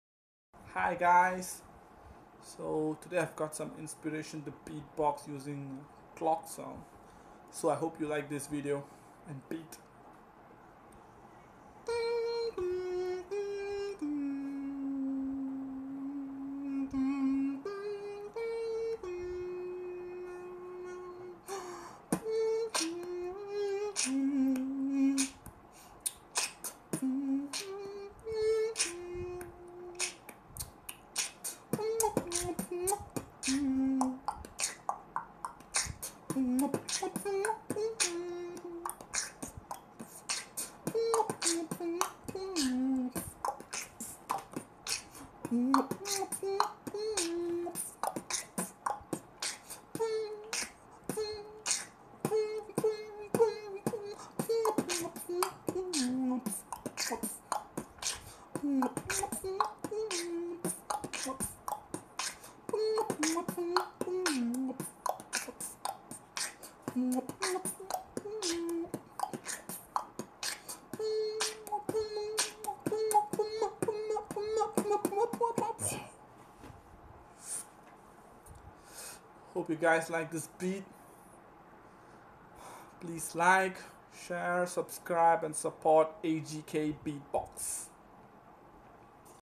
Freestyle Beatbox - Clock Chime | AGK Beatboxer
Got inspired when my Westminster wall clock ringed at 12pm and decided to make this beatbox video